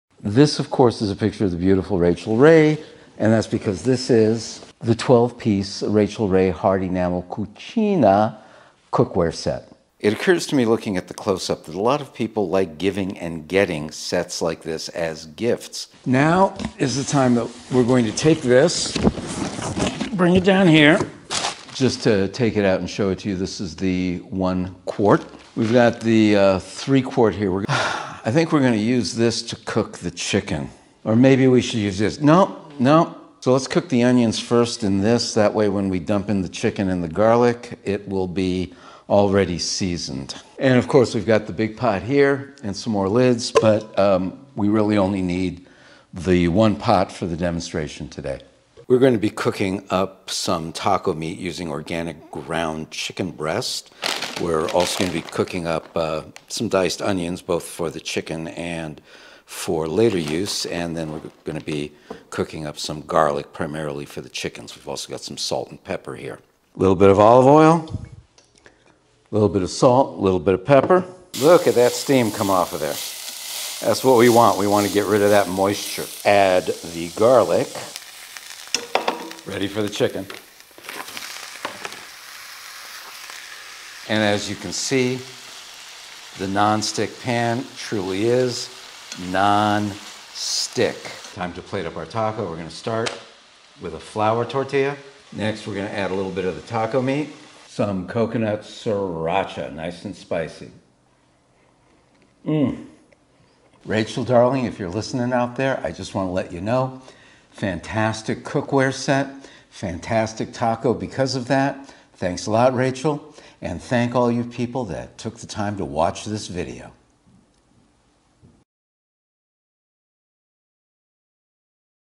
Rachael Ray Cucina Nonstick Cookware Pots and Pans Set, 12 Piece
Nonstick pots and pans from Rachael Ray are crafted with durable aluminum and sturdy hard enamel porcelain exteriors. SET INCLUDES: 1-quart and 3-quart saucepans with lids, 6-quart stockpot with lid, 8.5-inch and 10-inch frying pans, 3-quart sauté pan with lid, slotted turner and spoon.